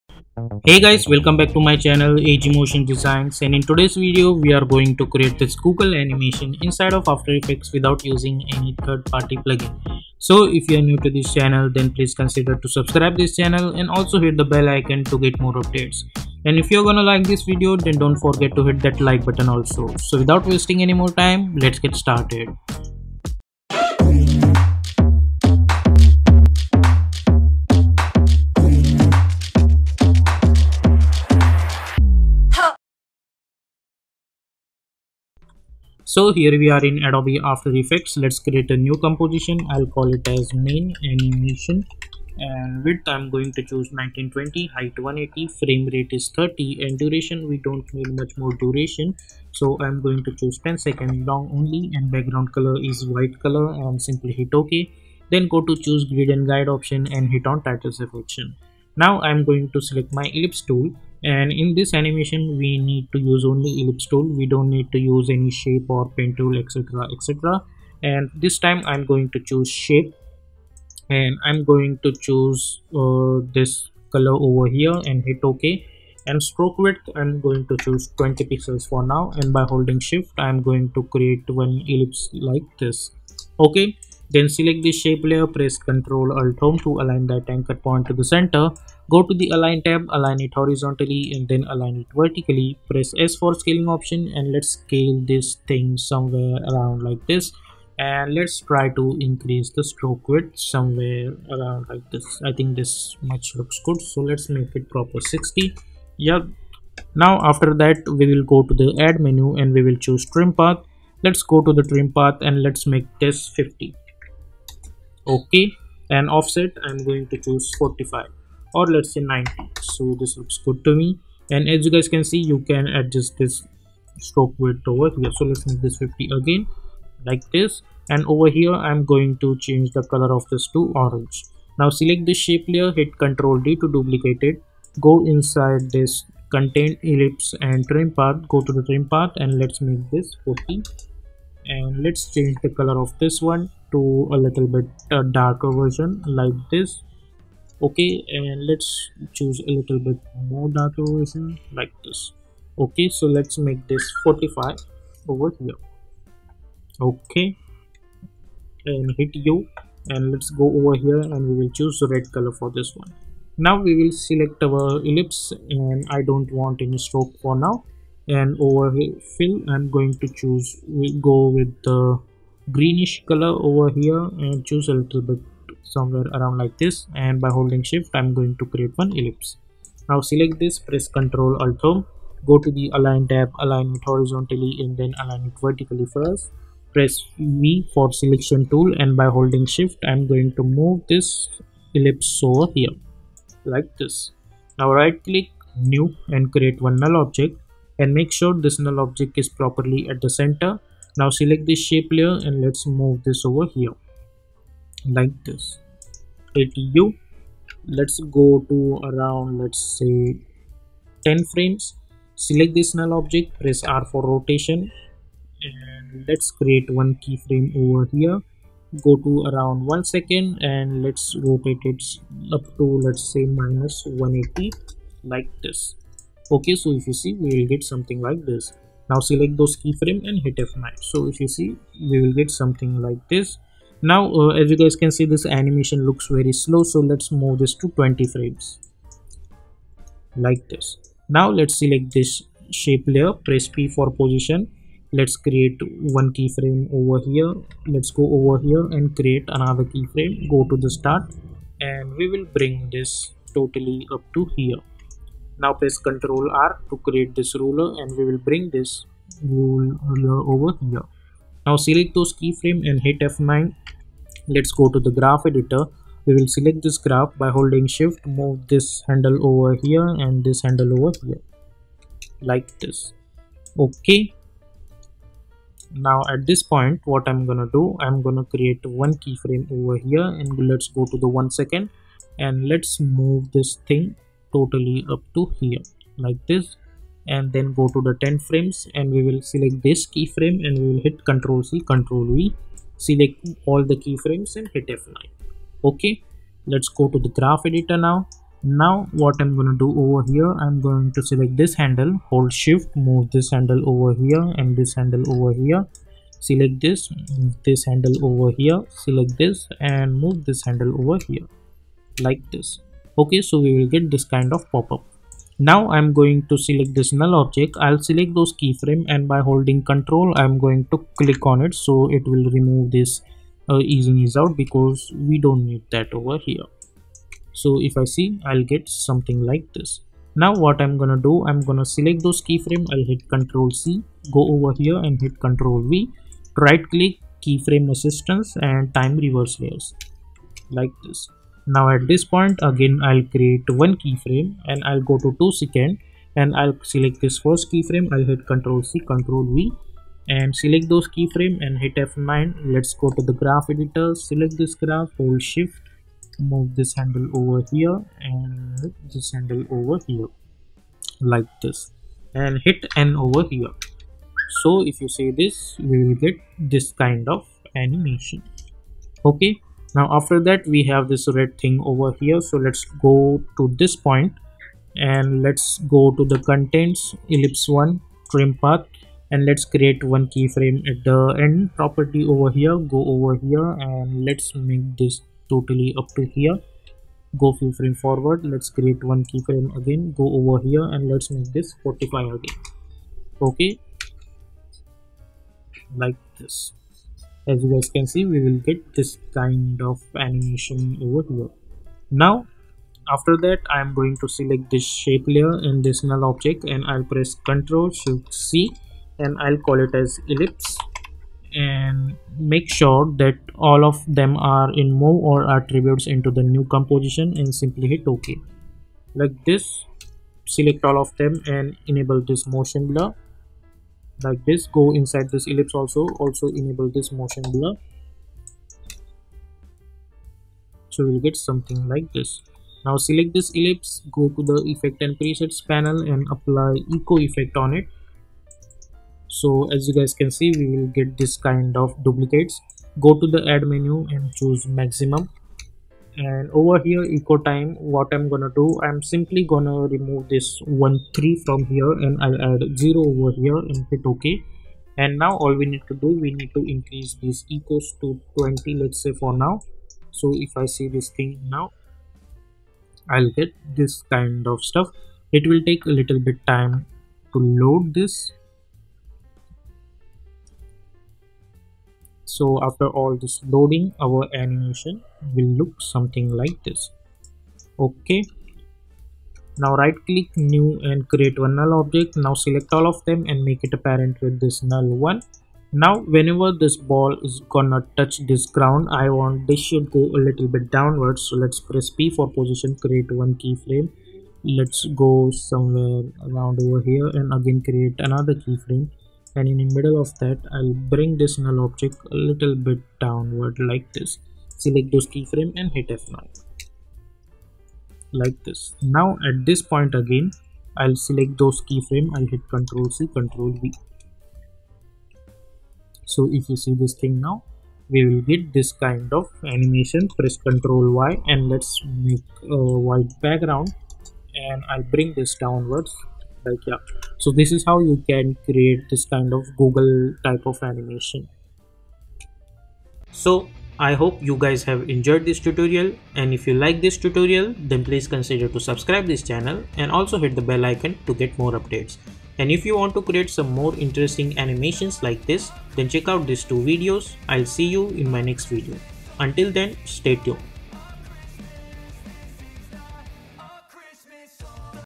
Google Animation In Adobe After Effects - After Effects Tutorial - No Plugins.
Google Animation In Adobe After Effects - After Effects Tutorial - Google Animations Tutorial.

Some of my best logo animation tutorials.

I hope you guys will enjoy this tutorial and I hope this tutorial will be helpful to improve your motion graphics skill.